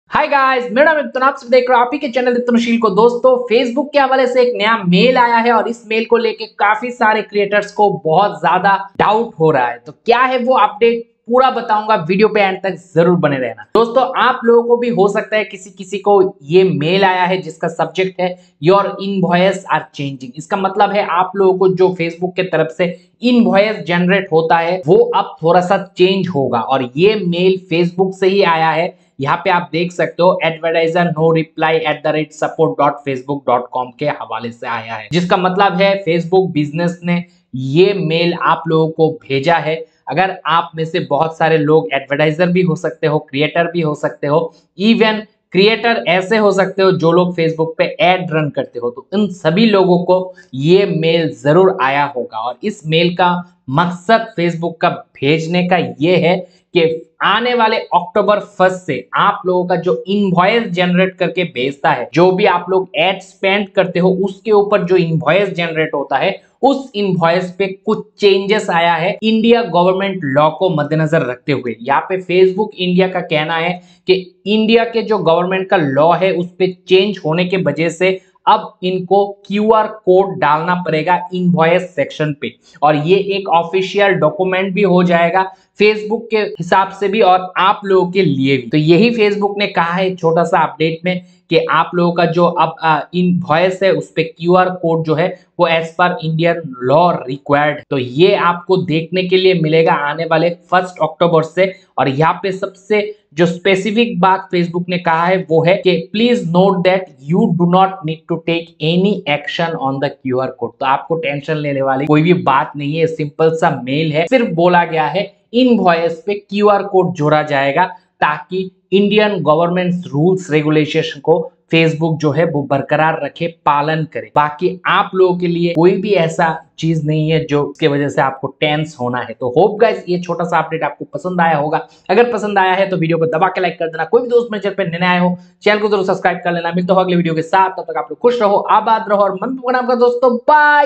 Facebook India Update | Facebook Ads Update | Facebook Business Update: Your Invoices Are Changing
Facebook India Update | Facebook Ads Update | Facebook Business Update: Your Invoices Are Changing // There are coming changes in the Facebook India invoice process!